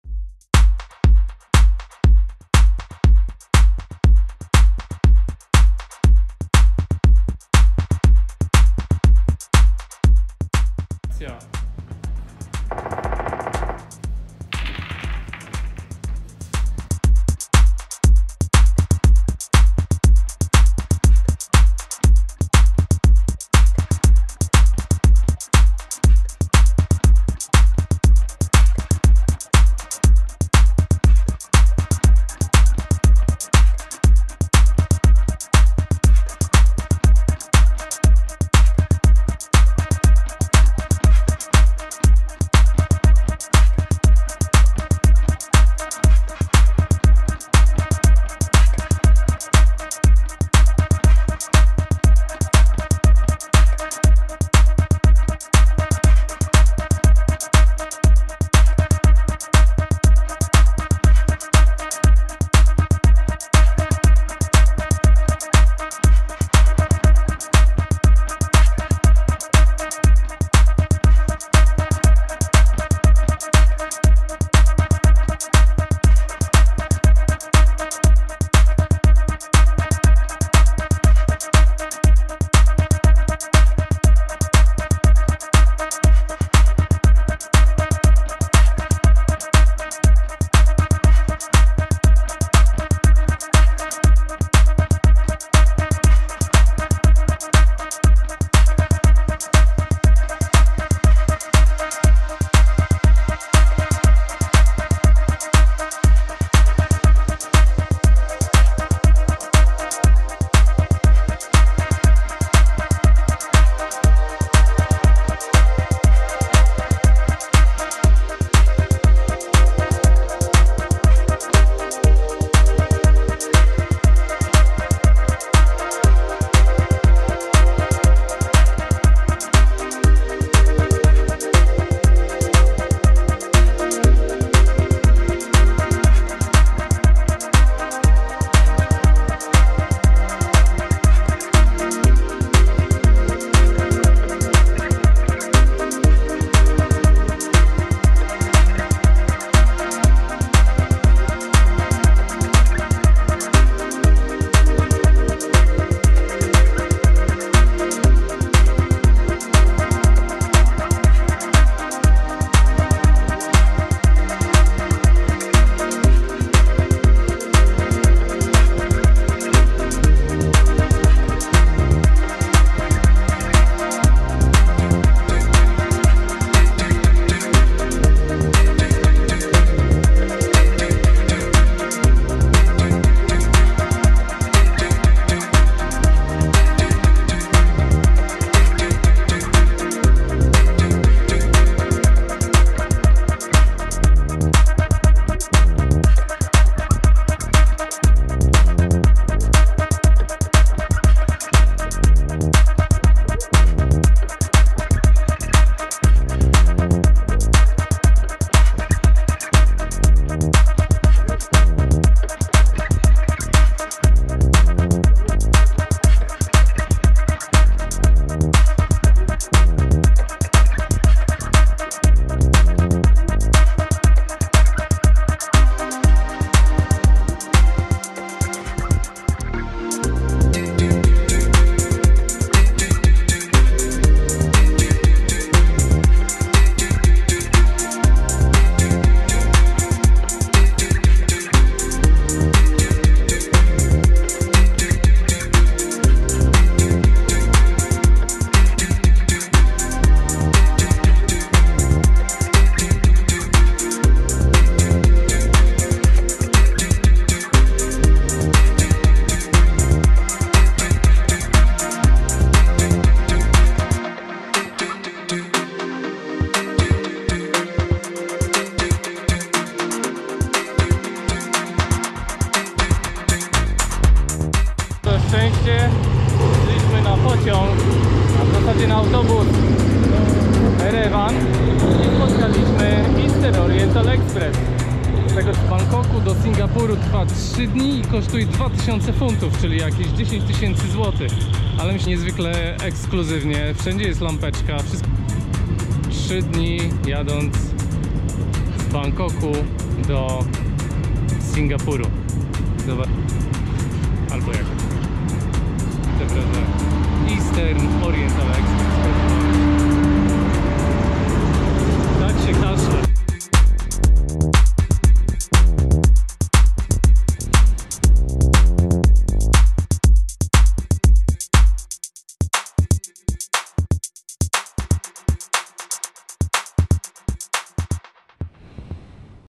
Tajlandia dzień #12-13 - w podróży z wyspy Koh Tao 🏝do Kanchanaburi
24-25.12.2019

Bilety na pociąg kupujemy przez stronę , kosztują 2 x drożej niż na miejscu ale przynajmniej mamy pewność że będziemy mieli miejsca obok siebie.